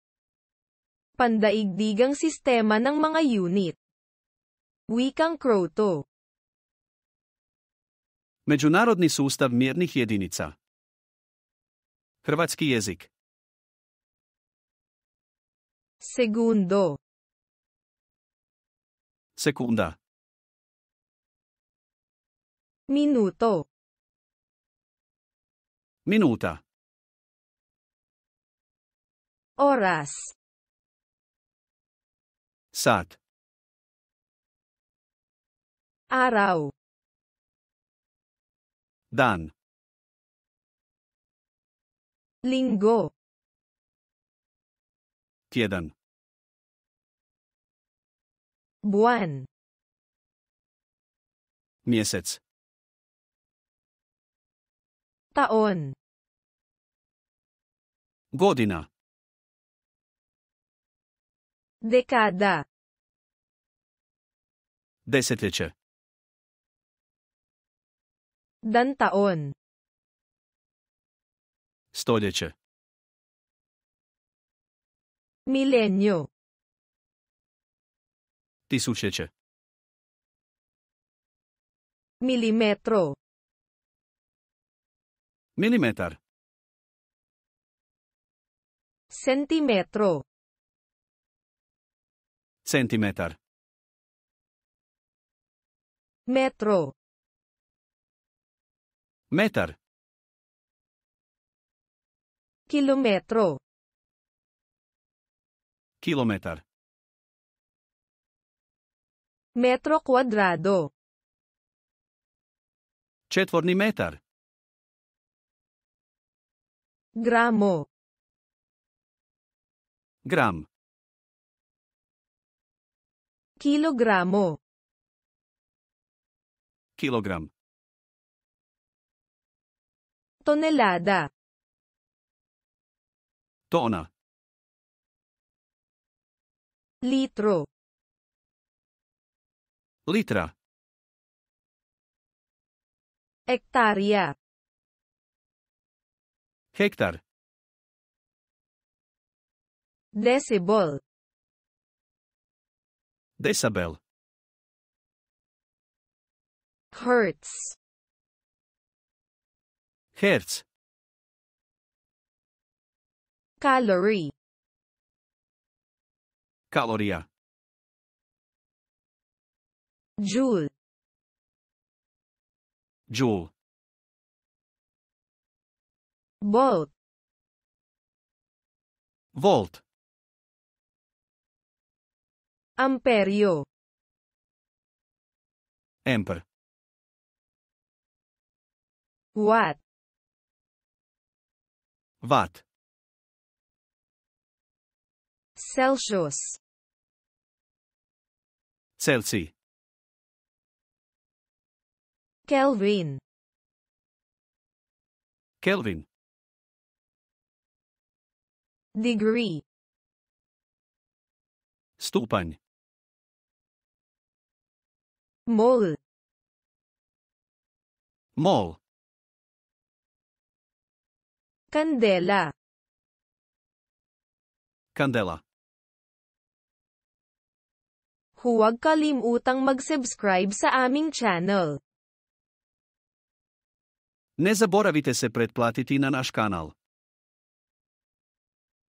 Pandaigdigang Sistema ng mga Yunit (Wikang Kroato) (tl-hr)
Fone TV
tl-hr-34
Međunarodni sustav mjernih jedinica (Hrvatski jezik)
Segundo - Sekunda
Minuto - Minuta
Oras - Sat
Araw - Dan
Linggo - Tjedan
Buwan - Mjesec
Taon - Godina
Dekada - Desetljeće
Dantaon - Stoljeće
Milenyo - Tisućljeće
Milimetro - Milimetar
Sentimetro - Centimetar
Metro - Metar
Kilometro - Kilometar
Metro kwadrado - Četvorni metar
Gramo - Gram
Kilogramo - Kilogram
Tonelada - Tona
Litro - Litra
Ektarya - Hektar
Decibel - Decibel
Hertz - Herc
Calorie - Kalorija
Joule - Džul
Bolt - Volt
Amperyo - Amper
Watt - Vat
Celsius - Celzij
Kelvin - Kelvin
Degree - Stupanj
Mole - Mol
Candela - Kandela